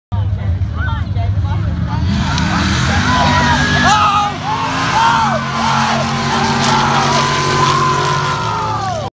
Newtown dragway
87 f150 vs square body Chevy